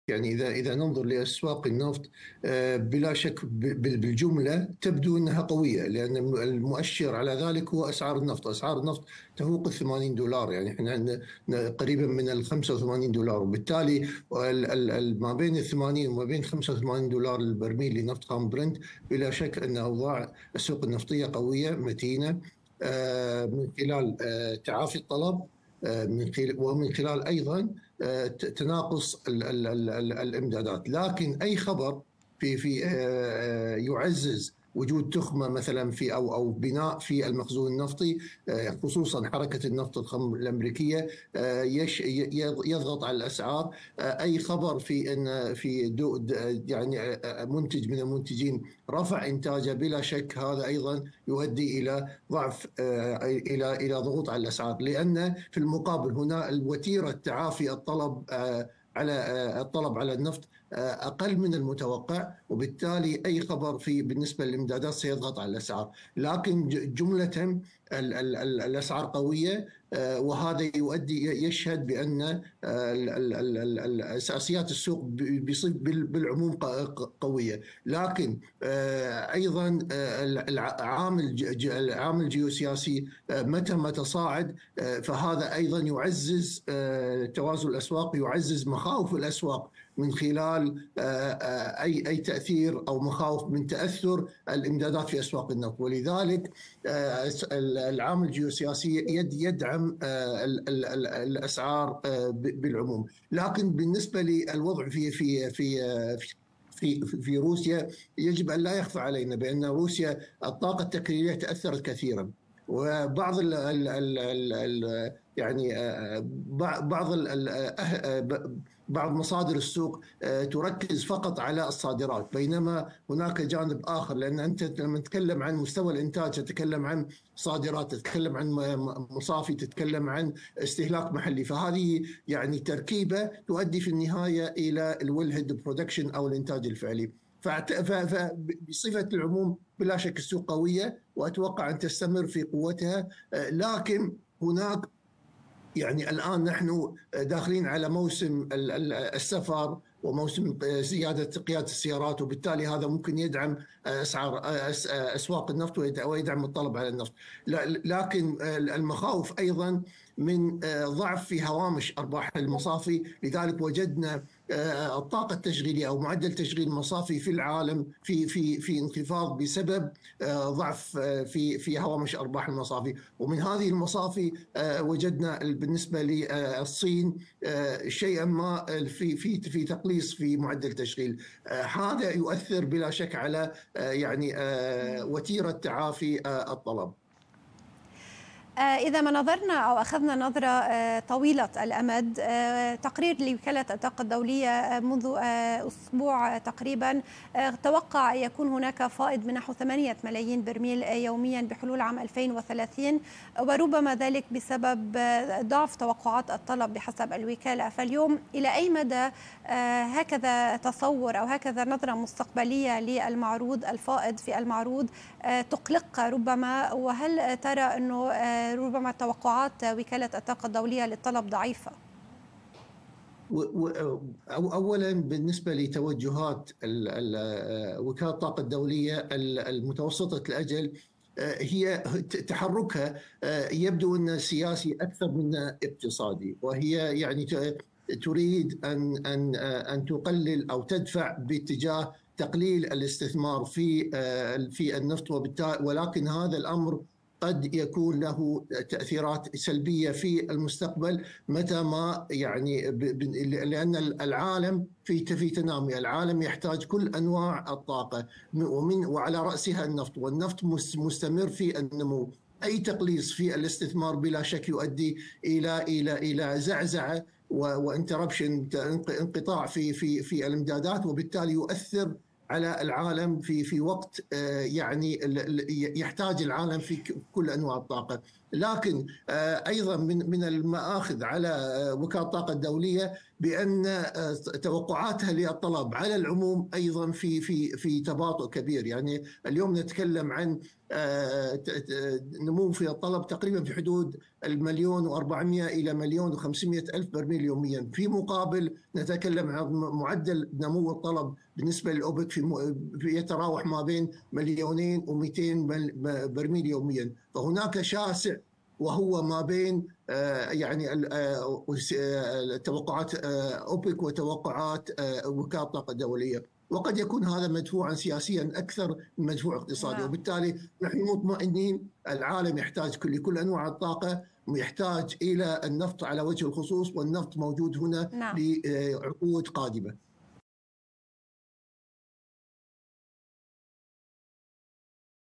محمد الشطي: توجهات وكالة الطاقة الدولية سياسية أكثر من أنها اقتصادية
◀️ الخبير النفطي محمد الشطي:
📌 هذه هي العوامل التي تؤثر على أسعار
📌 توجهات وكالة_الطاقة_الدولية سياسية أكثر من أنها اقتصادية، وتوقعاتها على الطلب في تباطؤ كبير